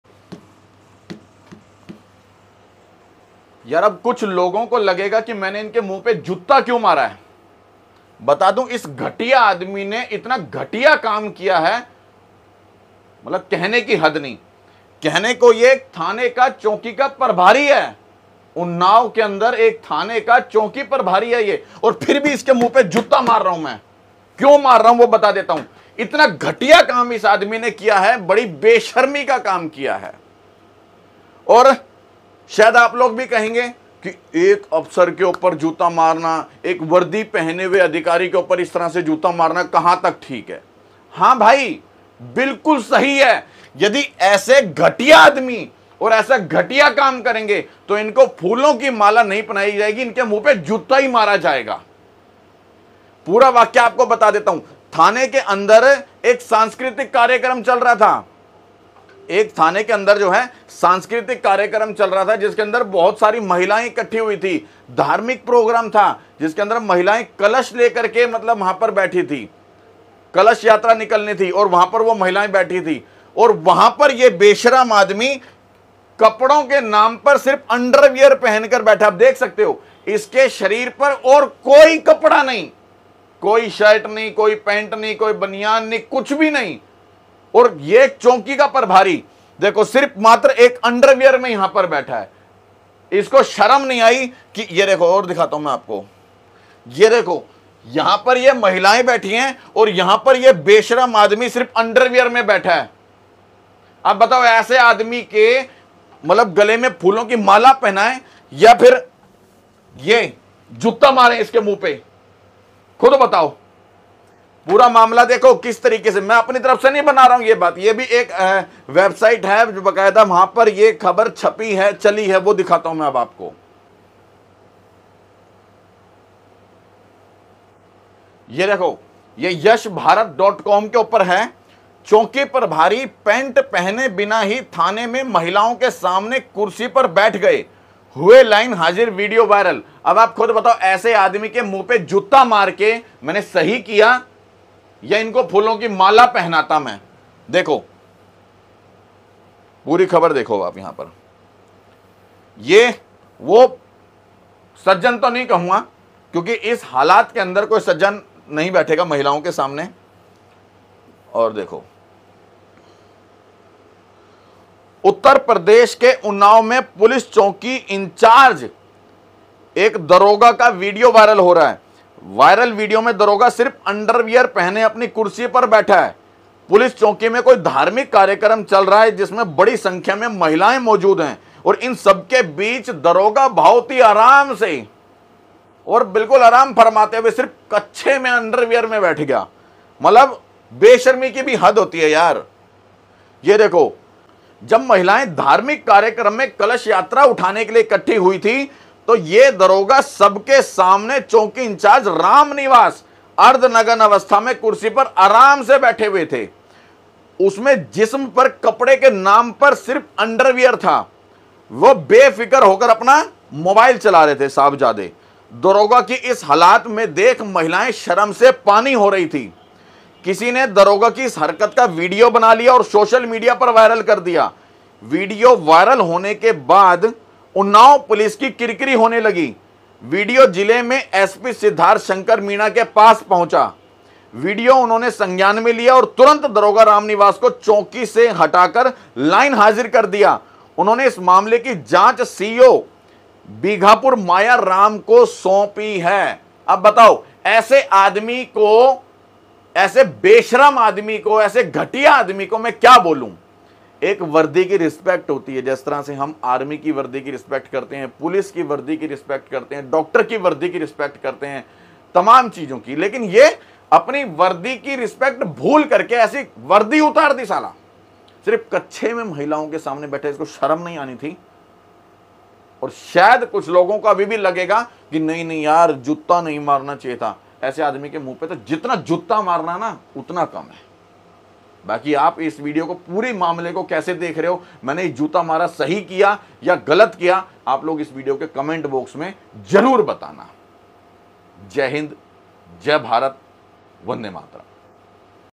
चौकी प्रभारी पेंट पहने बिना ही थाने में महिलाओं के सामने कुर्सी पर बैठ गए
यदि आपको लगता है हम इस चैनल पर जनहित पर अच्छे मुद्दे उठाते है तो आप हमें सपोर्ट जरूर करें, चैनल सब्सक्राइब करें और वीडियो को आगे शेयर भी जरूर करें🙏🙏